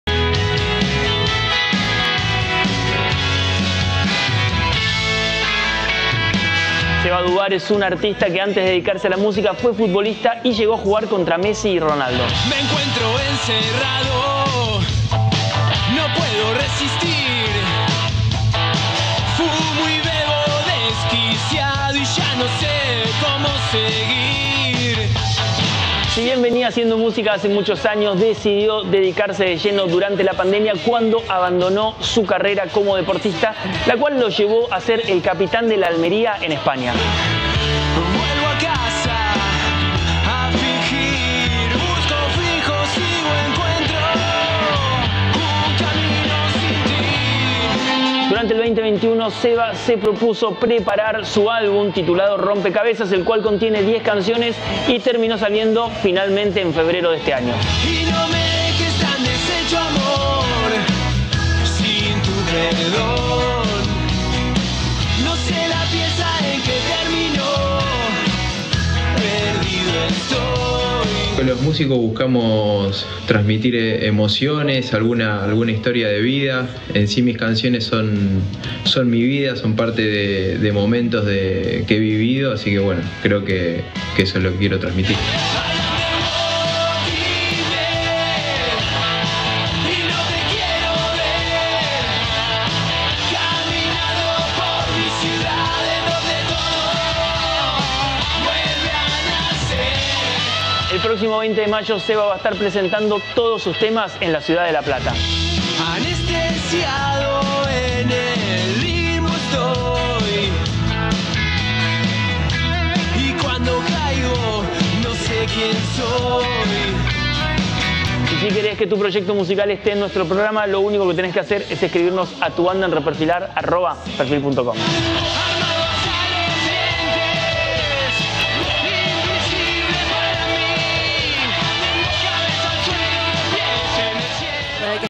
Tu Música en RePerfilAr: Seba Dubar, un artista con mucha explosión
El próximo 20 de mayo el artista estará presentando todos sus temas en la ciudad de La Plata.